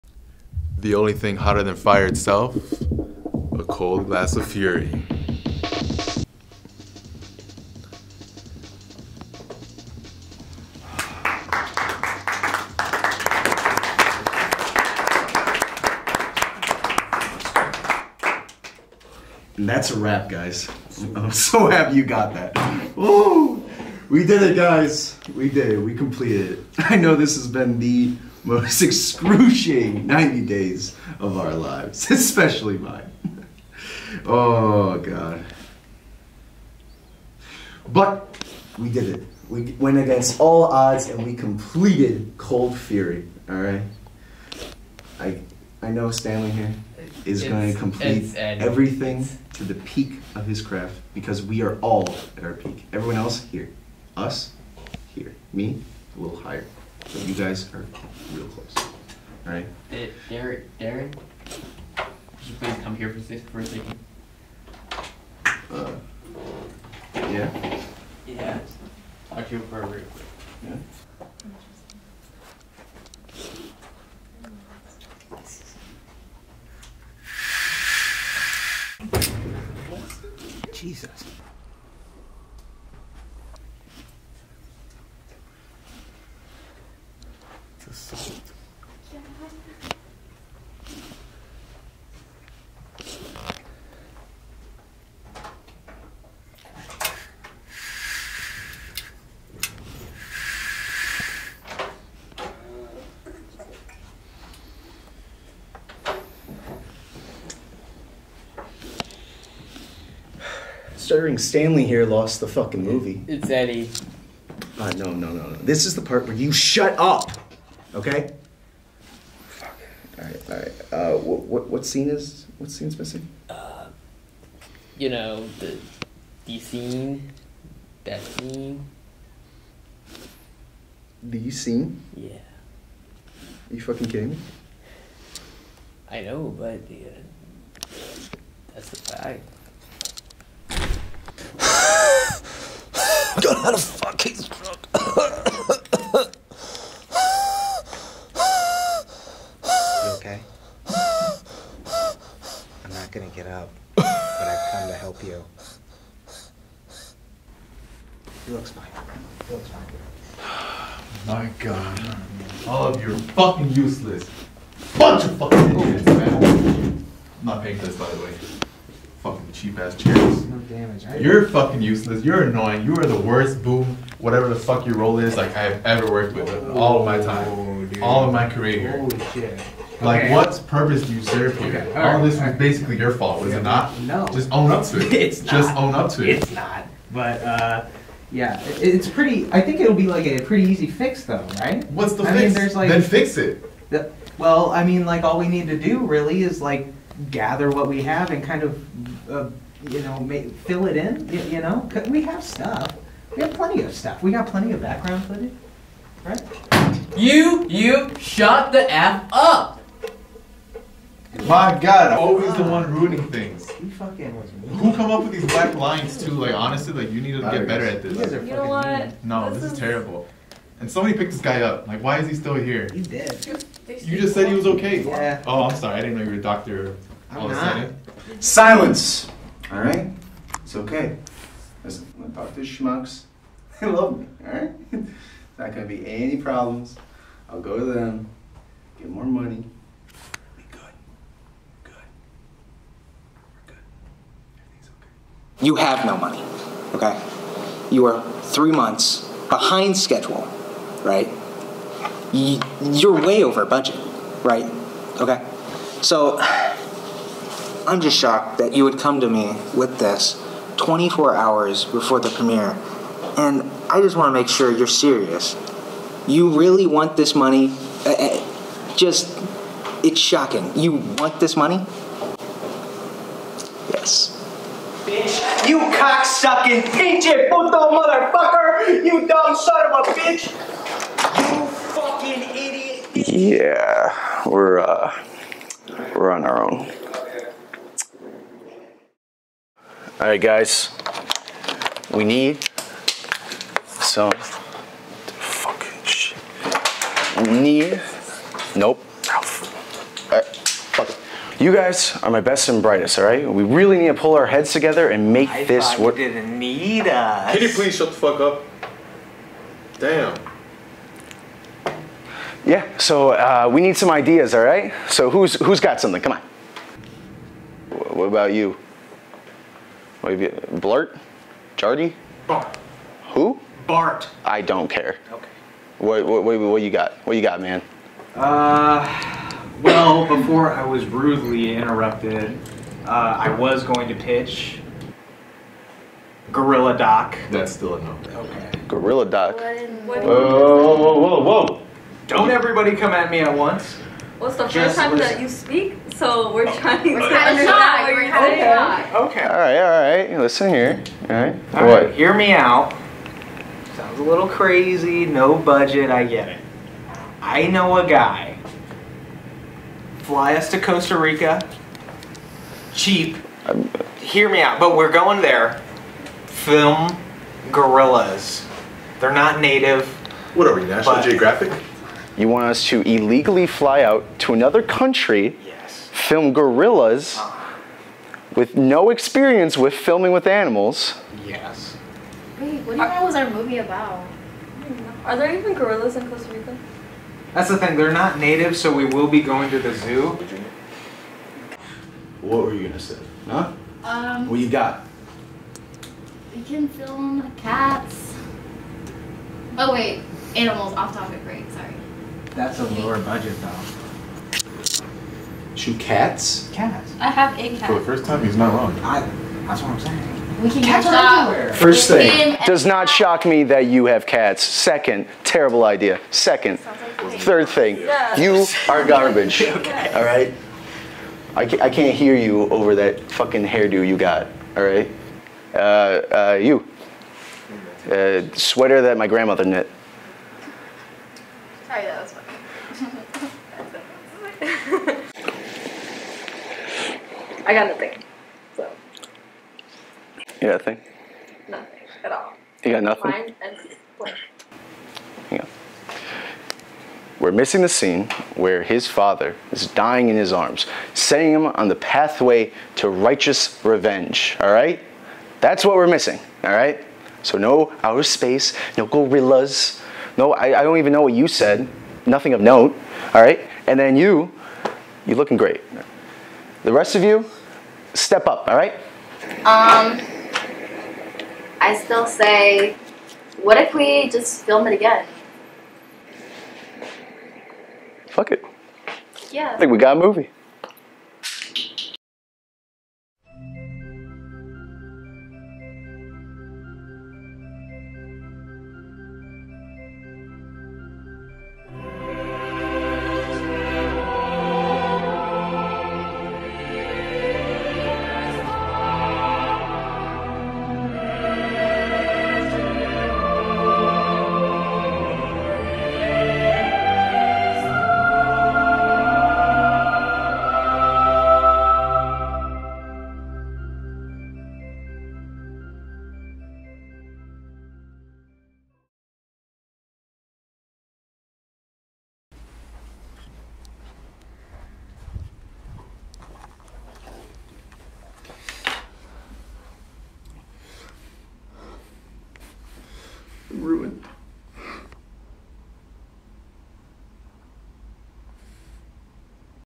Ready, Set, Action! - Comedy Short Film
Follow a Film Crew of the future hit film "Cold Fury" rushing to complete the film when it all goes wrong. Special thanks to my amazing crew everyone played their roles perfectly on camera and off. It was an incredible shoot with an incredible crew. Now I hope you laugh, cry, and feel all the emotions, without further ado Mouthful Productions presents READY, SET, ACTION!